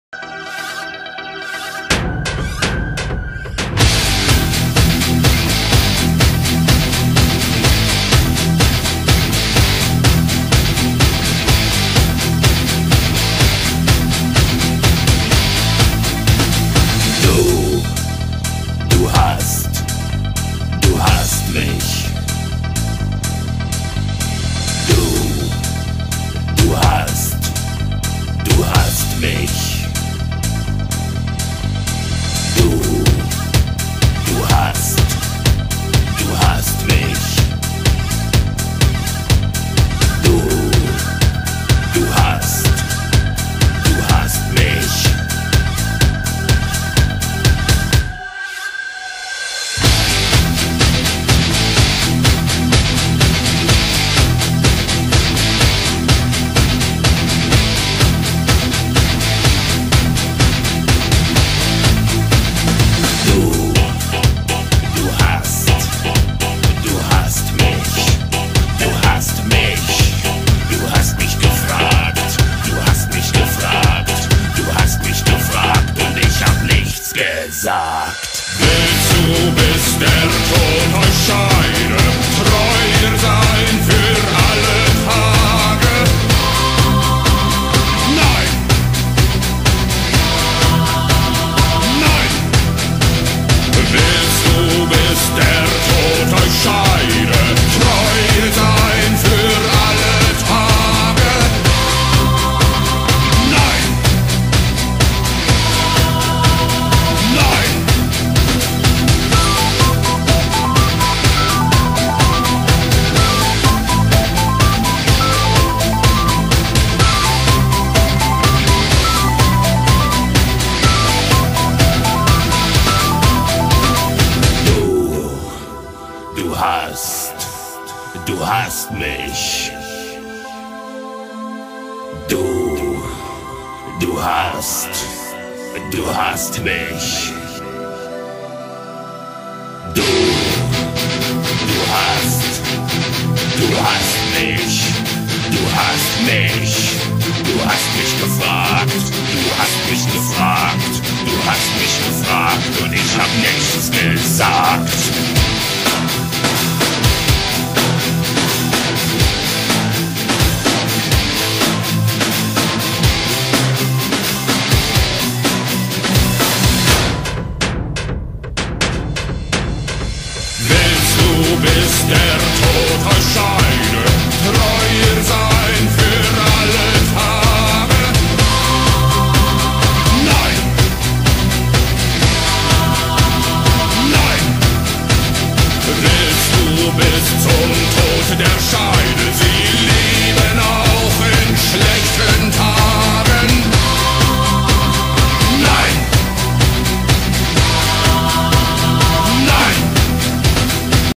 Cat Movie Madness
The effects of combining cat macro pics, Rammstein music, and Windows Movie Maker! Yay Chaos!